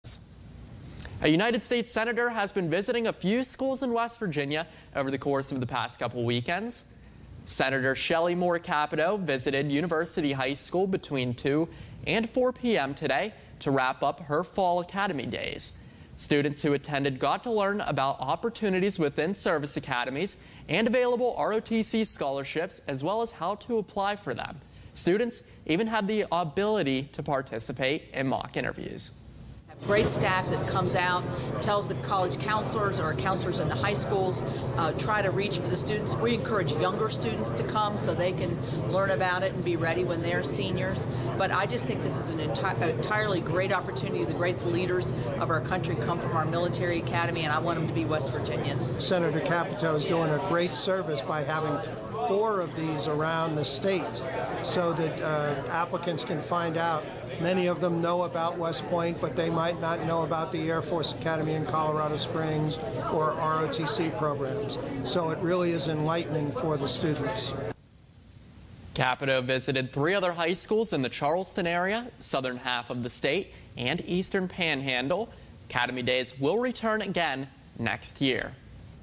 WBOY: Capito Hosts Academy Day Event at University High School
U.S. Senator Shelley Moore Capito hosts her fourth Academy Day event of the fall at University High School in Morgantown.